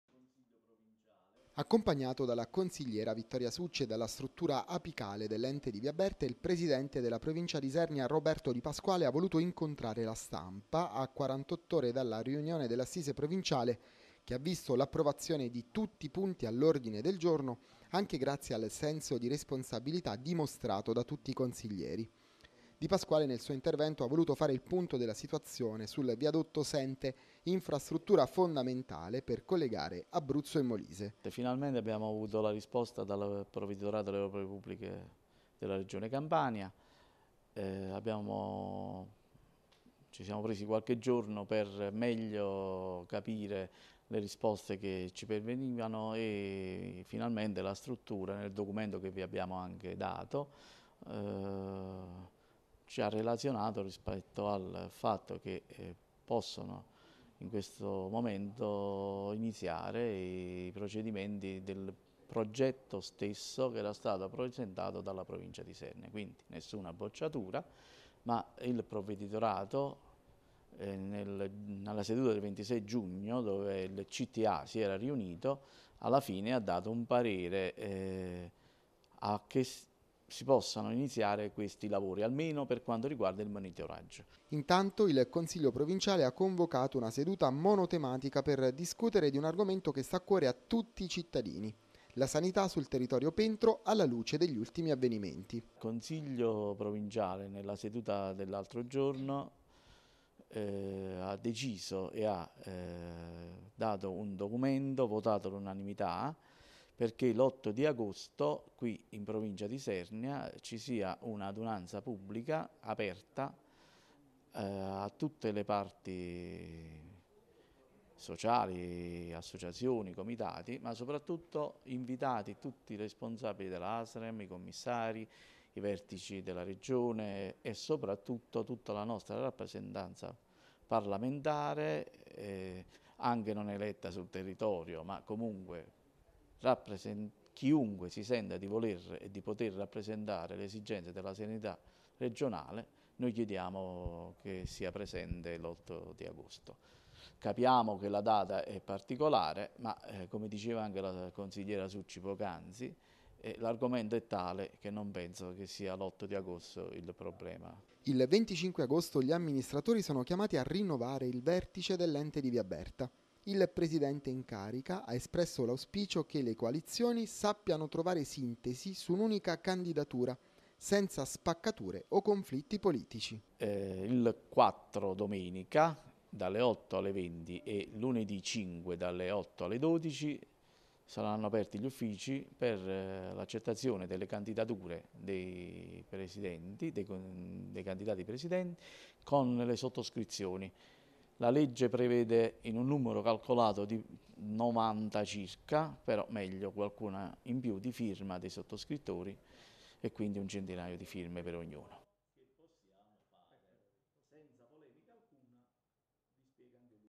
Il presidente della Provincia di Isernia a 360° su elezioni, Sente e sanità - ...
Servizi TG | Il presidente della Provincia di Isernia a 360° su elezioni, Sente e sanità | 31/7/2019